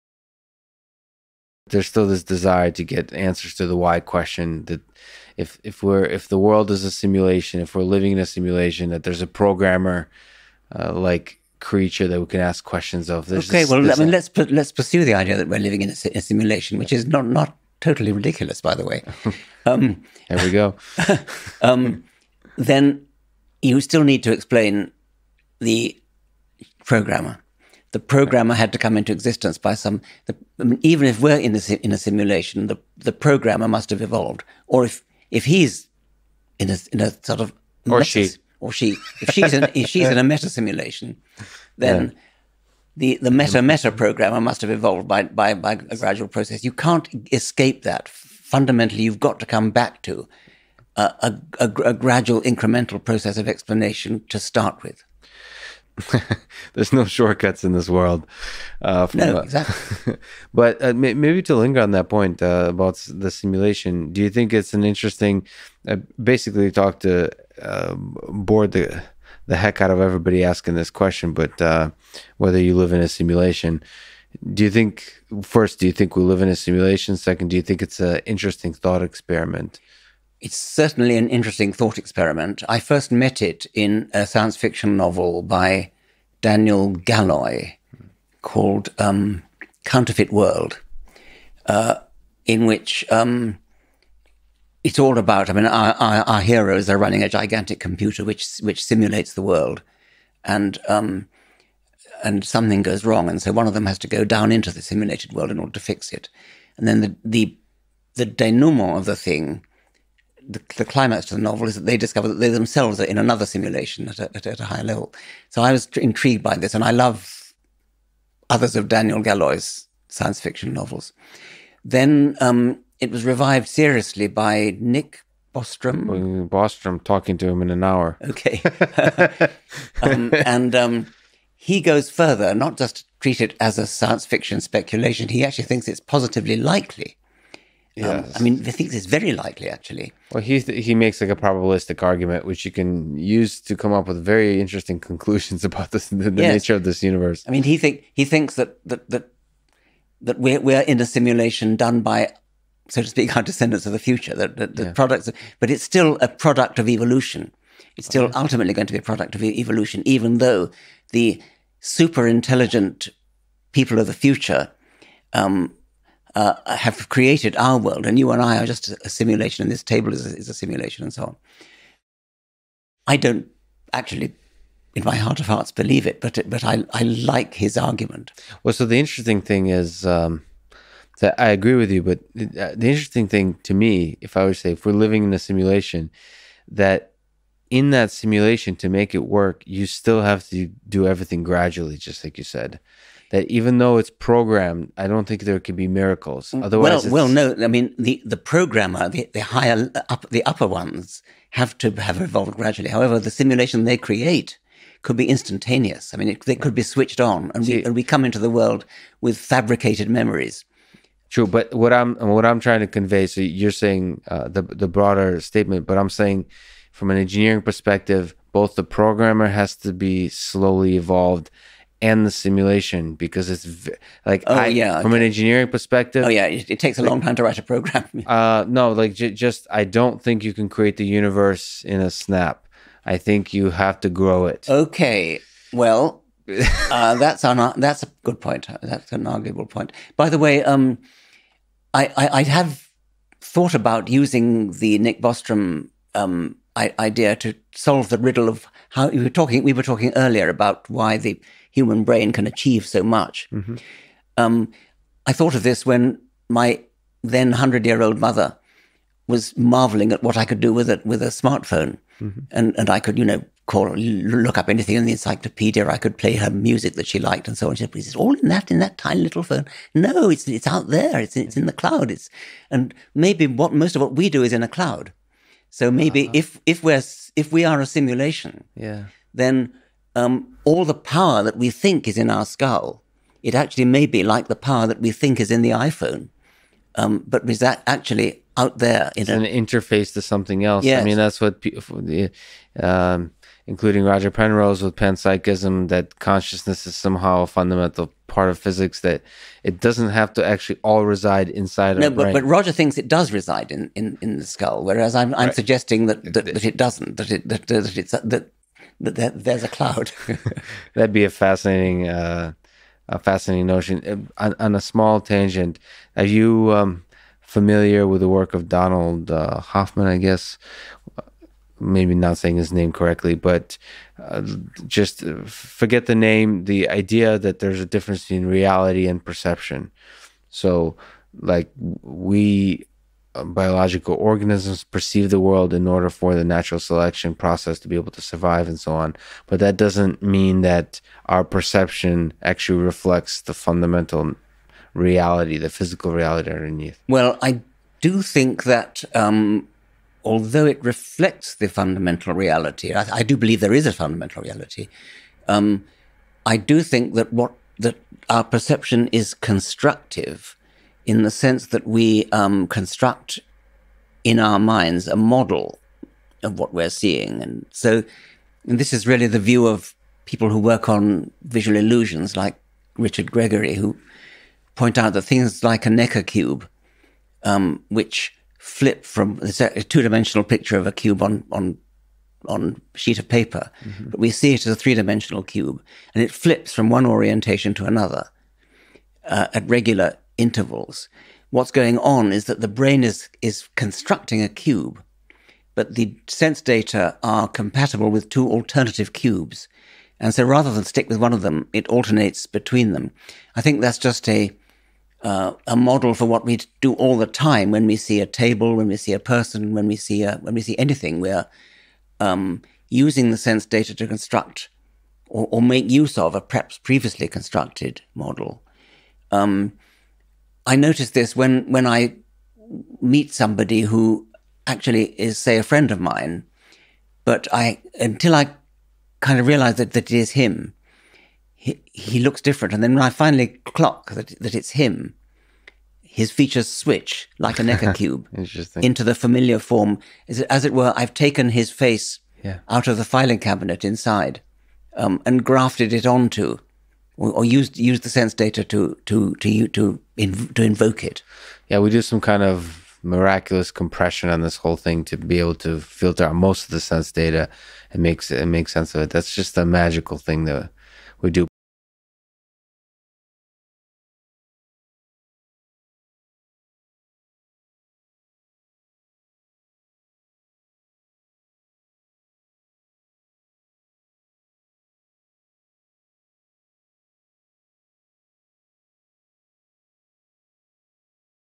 Richard Dawkins: The Programmer of the Simulation Came About Through Evolution | AI Podcast Clips
Richard Dawkins is an evolutionary biologist, and author of The Selfish Gene, The Blind Watchmaker, The God Delusion, The Magic of Reality, The Greatest Show on Earth, and his latest Outgrowing God. He is the originator and popularizer of a lot of fascinating ideas in evolutionary biology and science in general, including funny enough the introduction of the word meme in his 1976 book The Selfish Gene, which in the context of a gene-centered view of evolution is an exceptionally powerful idea. He is outspoken, bold, and often fearless in his defense of science and reason, and in this way, is one of the most influential thinkers of our time.